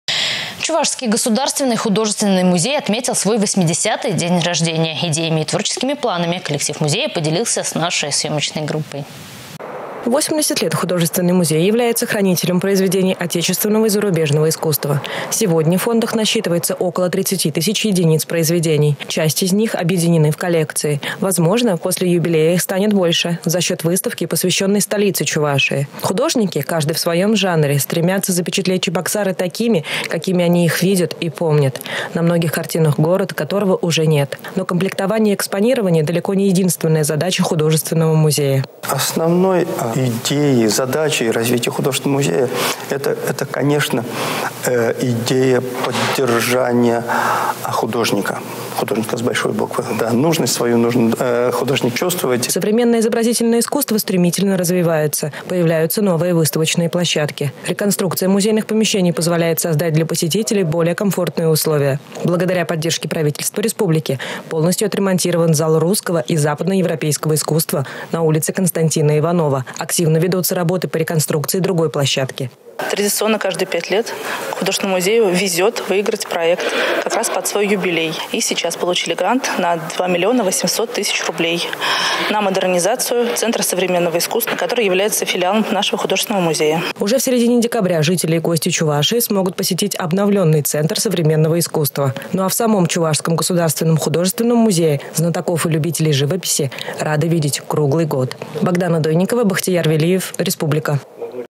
Чувашский государственный художественный музей отметил свой 80-ый день рождения
Чувашский государственный художественный музей отметил свой 80-ый день рождения.  Идеями и творческими планами коллектив музея поделился с нашей съемочной группой.